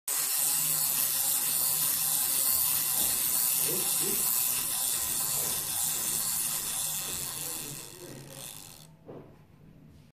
Dancing Robot
I found this toy on Hongkong Temple Street Night Market